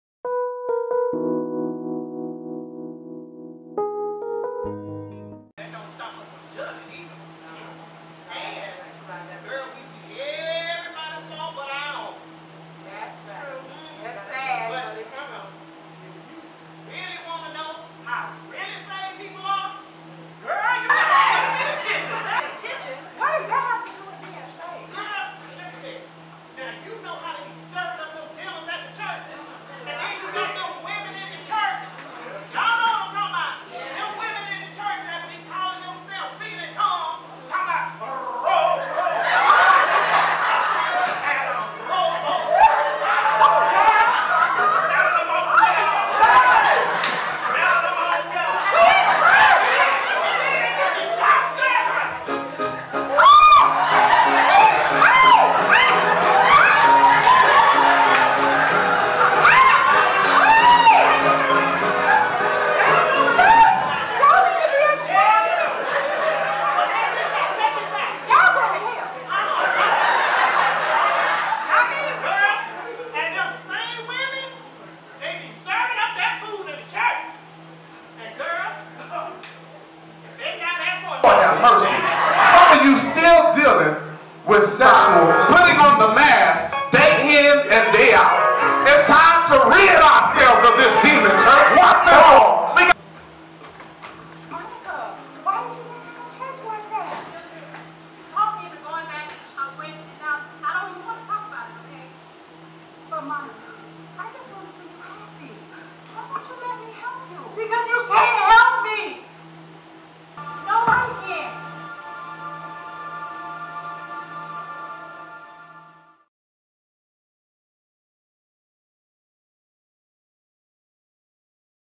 Prodigal Daughter, The Journey Home
A Gospel Stage Production that follows the life of one young lady who endured childhood bullying and abuse. After losing her way for many years as a result, she finally has the courage to go home..written by Actress Regina Hodges